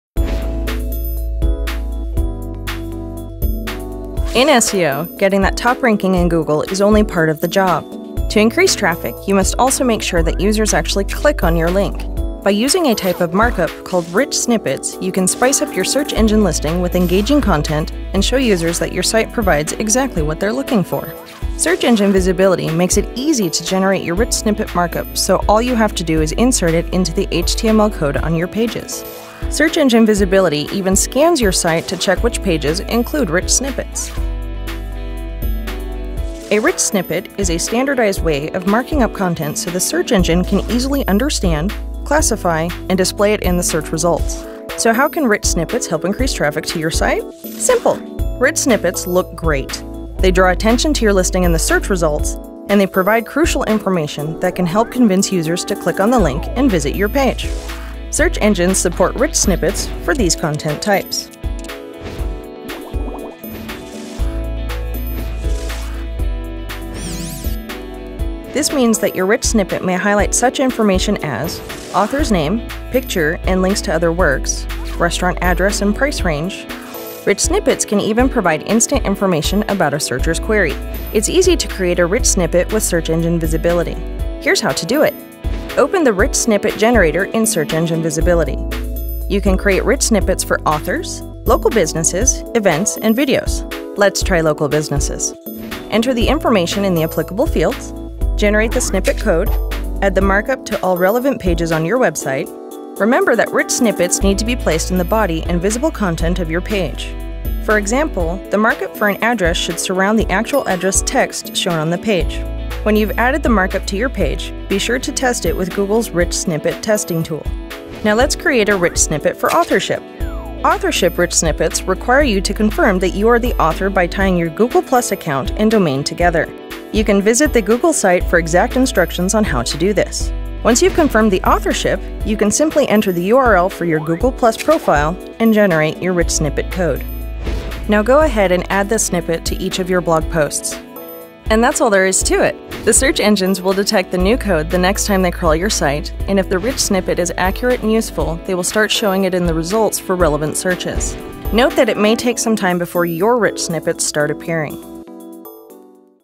Enhance your search engine ranking (seo) with rich snippets | GoDaddy
By adding rich snippets to your website, you can enhance your search engine listing and help increase click-through rates. Watch this tutorial and see how to create rich snippets for local businesses, authorship, events, and much more with GoDaddy's Search Engine Visibility SEO service. Optimize your website and draw traffic to your business with Search Engine Visibility from GoDaddy